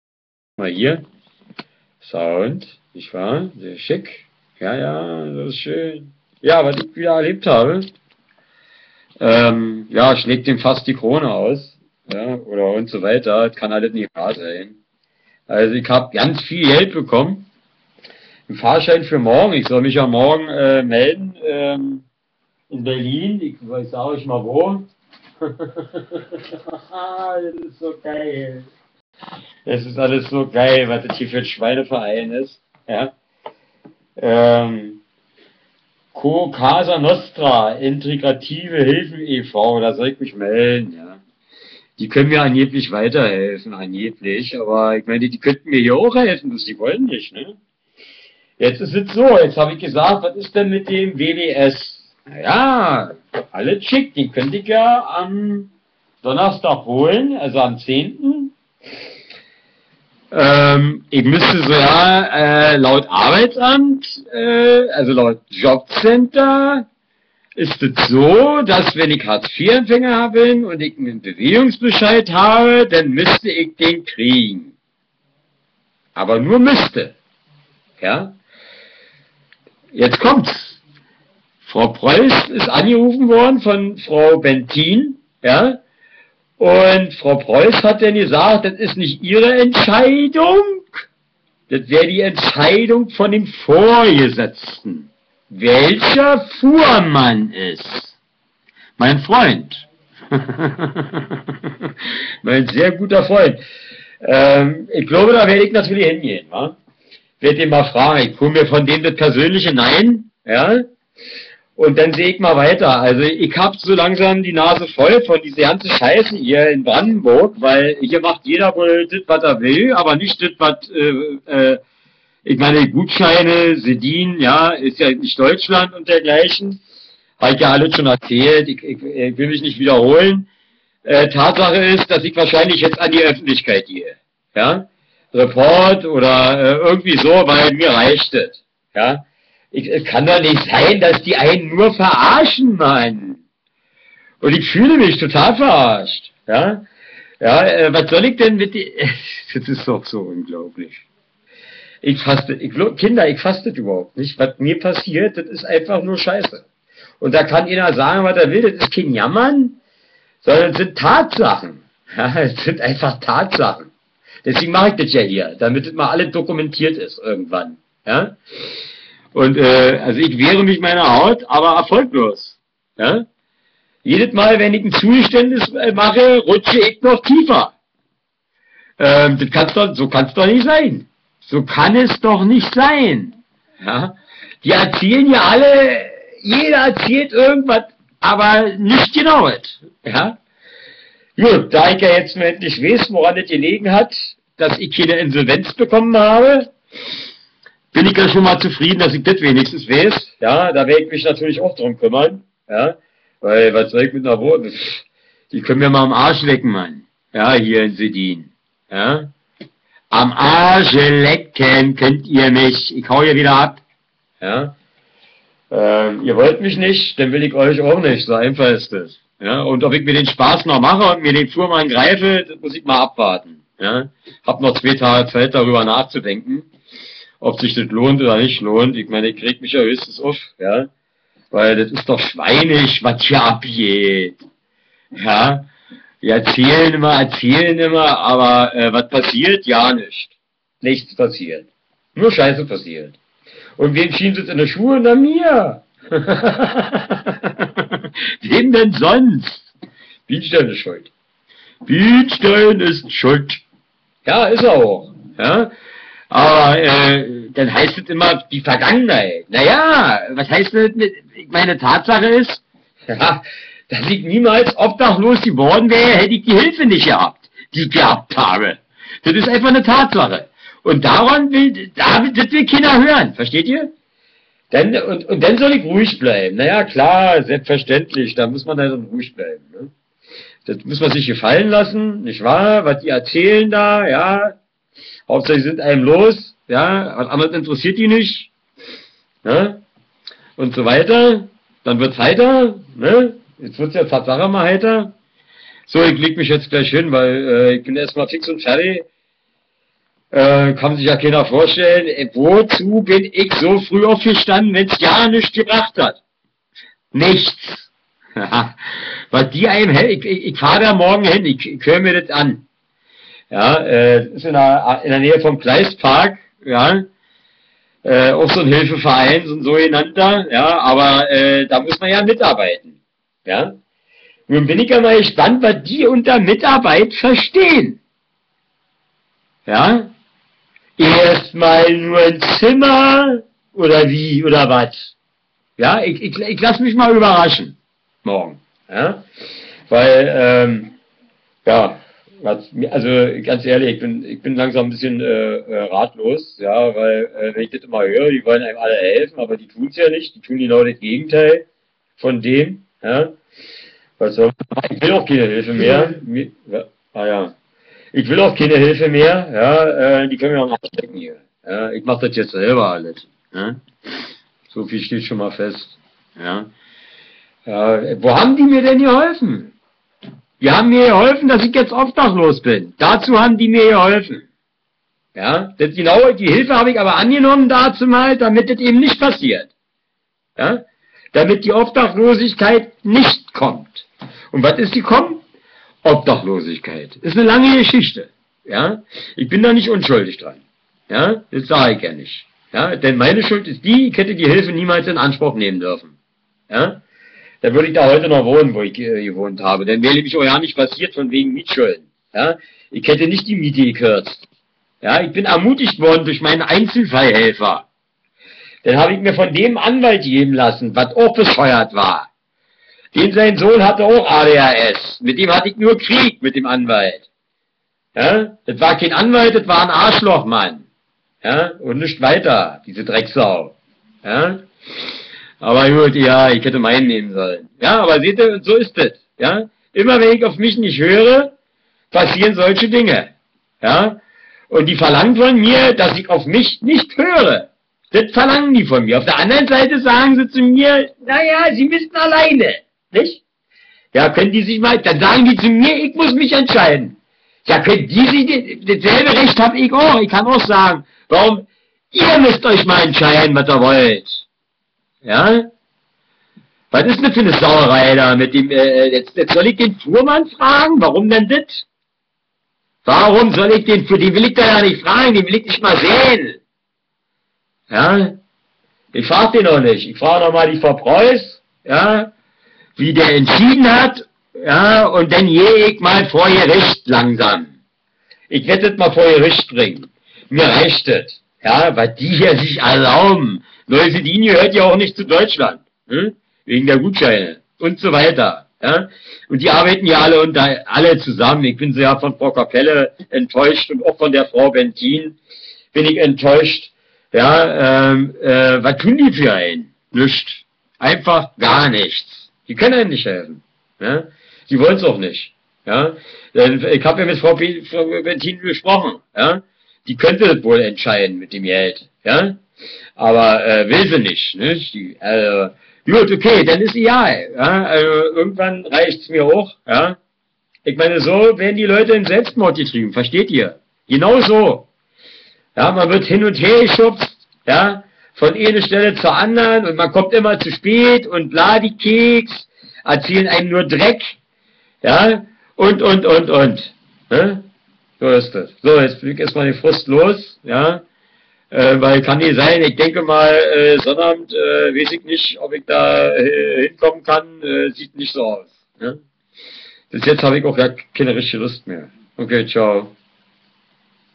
Webcam-Video vom 8. April 2014 14:02
Mr.Sound is echt sauer und überlegt An die Öffentlichkeit zu gehen oder auch ein Gericht anrufen was mir passiert schreit zum Himmel !!!